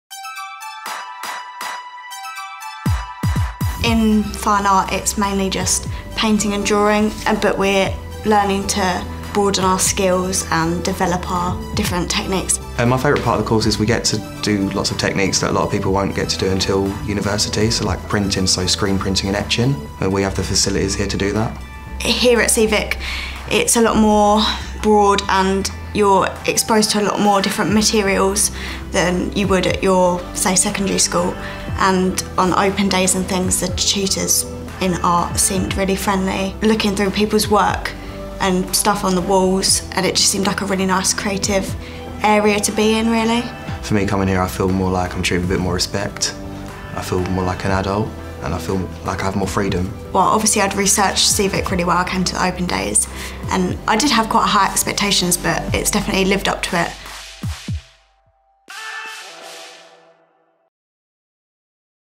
Fine Art at Seevic College
Students Helen and Tyler discuss the A-Level Fine Art at Seevic College, which will provide you with the opportunity to enhance your knowledge of the fundamental visual elements of representational art.